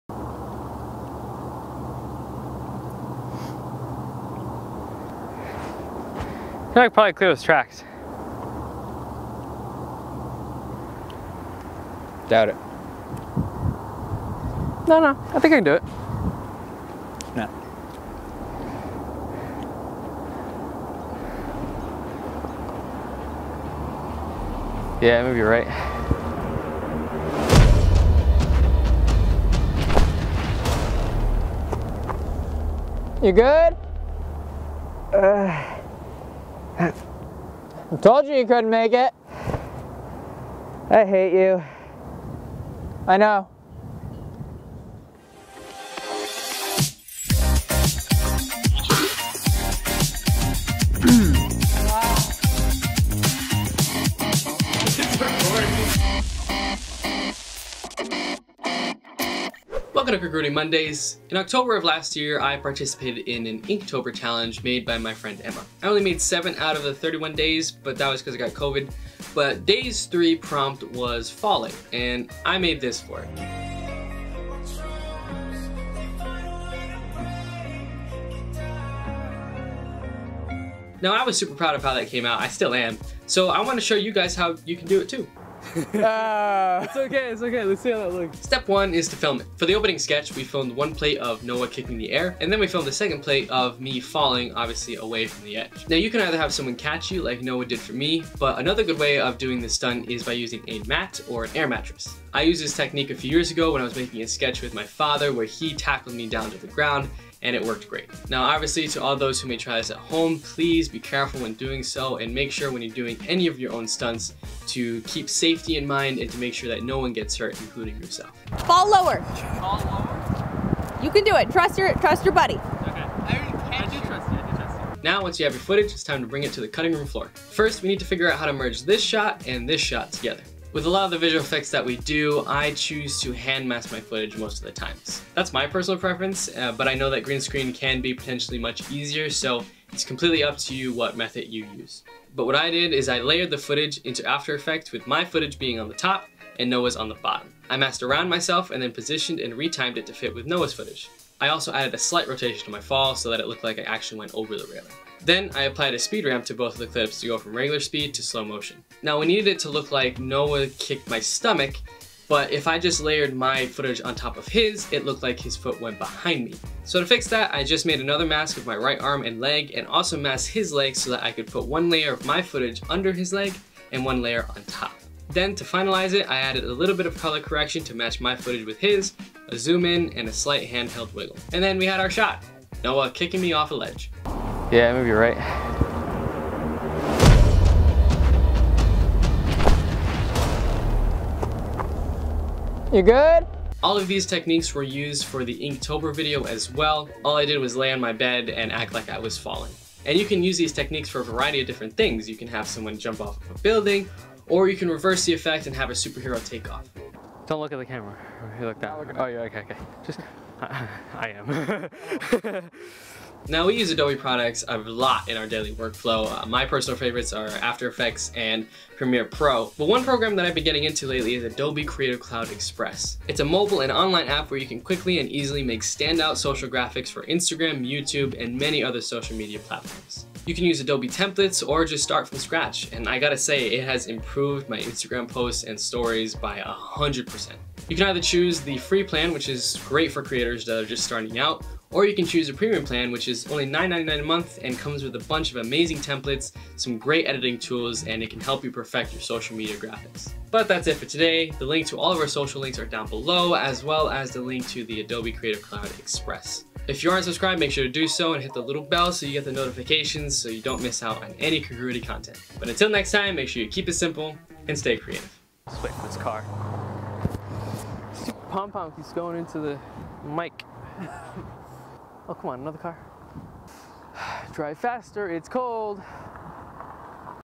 How to FAKE FALL off a Cliff Using After Effects | Mondays
Noah kicked me off a cliff for this video, but it's ok, we're still friends.

P.S. I didn't actually fall

0:00 - Top 10 Congruity Betrayals
0:47 - Intro
1:28 - How to Film It
2:05 - How to Edit It
3:33 - Adobe Creative Cloud Express
4:11 - Outro